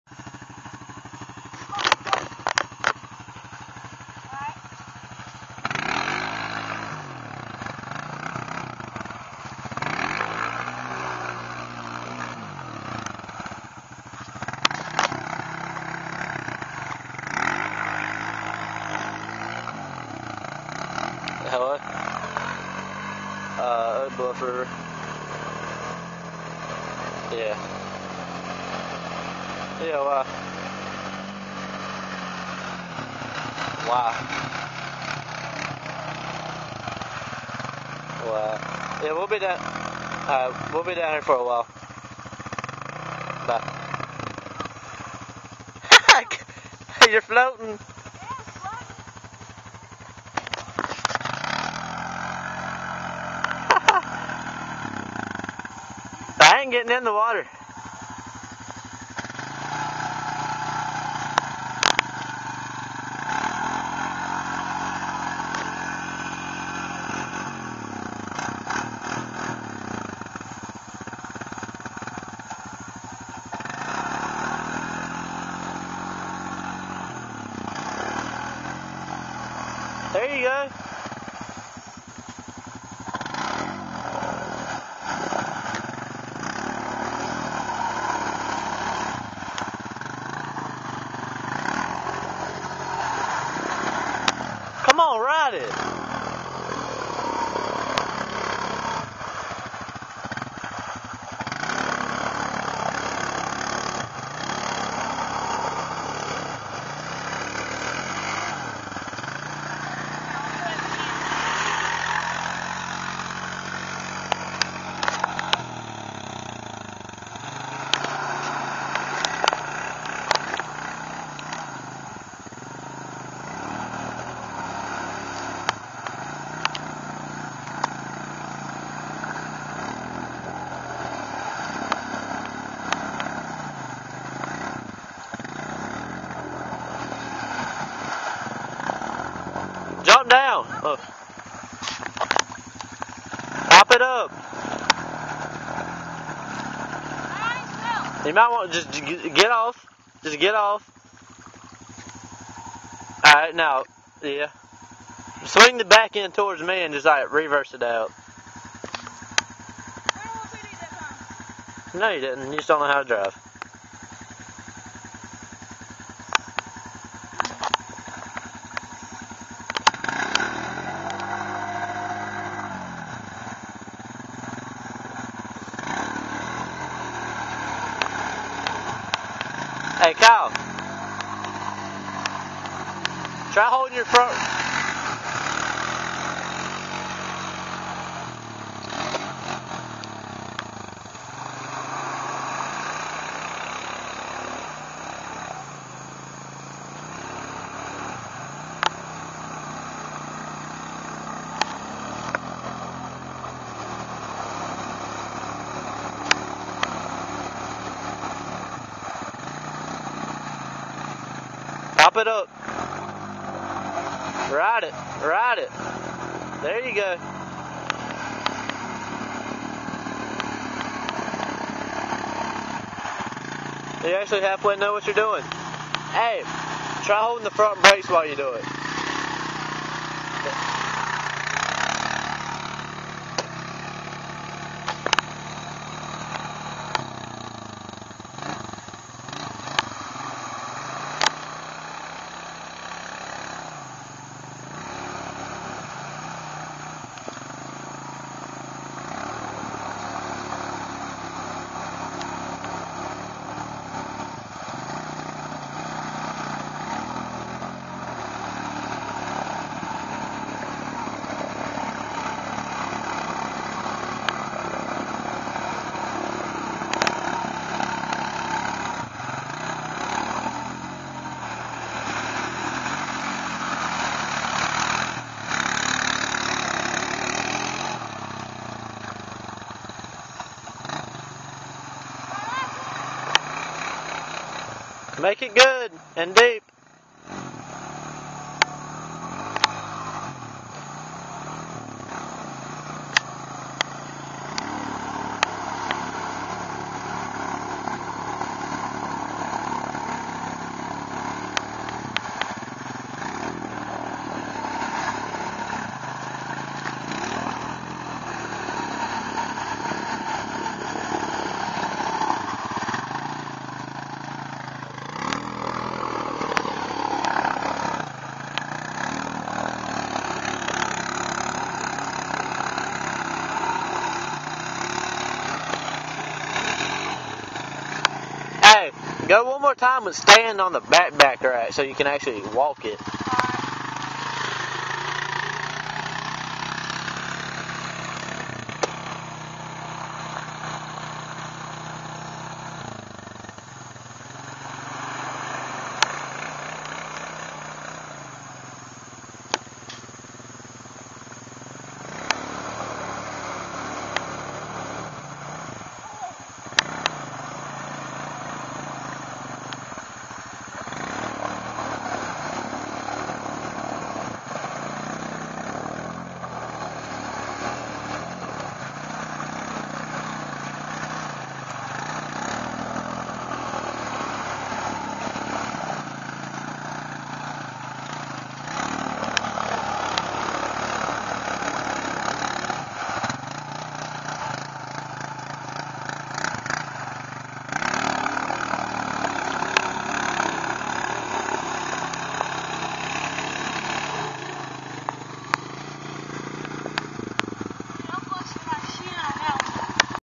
Rancher Water Wheelies
kyle owens ridin water wheelies at oakbluff